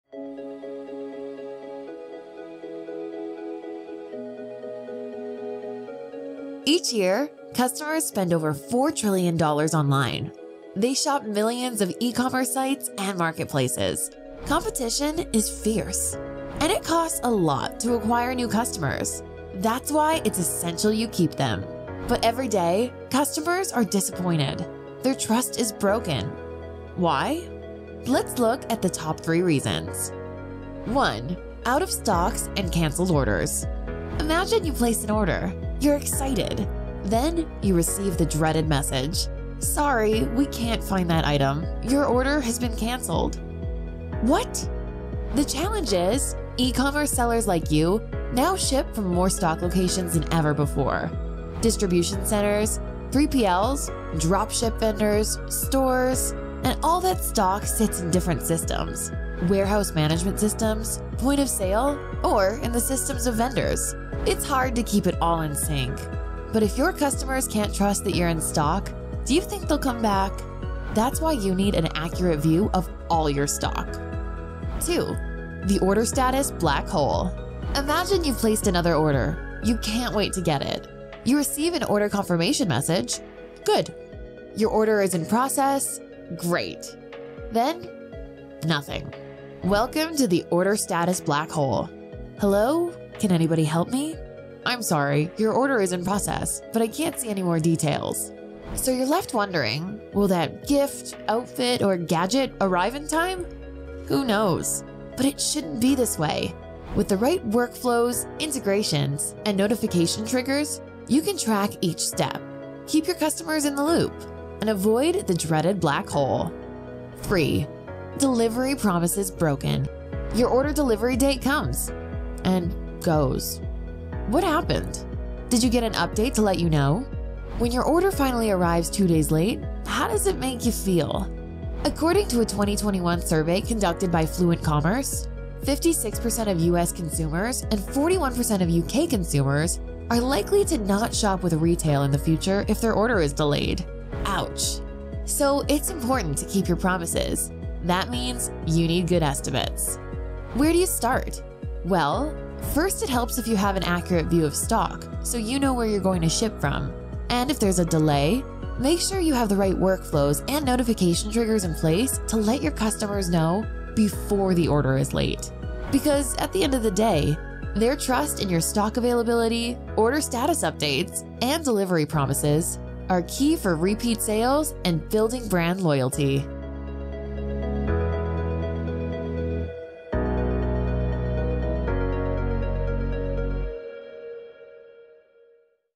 3 Ways Your Fulfillment CX can Build or Break Customer Trust | Fluent Commerce
Brand trust builds loyalty. And every step in the shopping experience is an opportunity to build
or break it. How does your fulfillment CX stack up? Watch this video to find out.